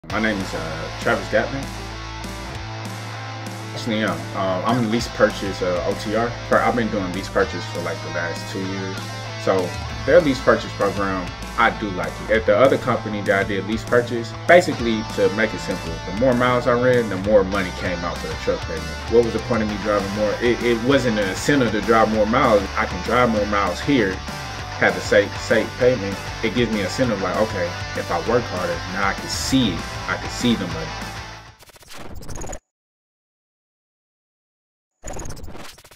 💥 Drive, Earn, and Thrive with Stevens Trucking! 💪
Hear it from one of our team members who’s been in the lease purchase program for 2 years: “If you work hard, you will see the money!” At Stevens Trucking, we believe in the work hard, play hard mentality. This is your chance to reach your goals, make the most of your career, and be part of a company that rewards your dedication! 🚛💸

Ready to take control of your future? Join Stevens Trucking and start driving toward success today!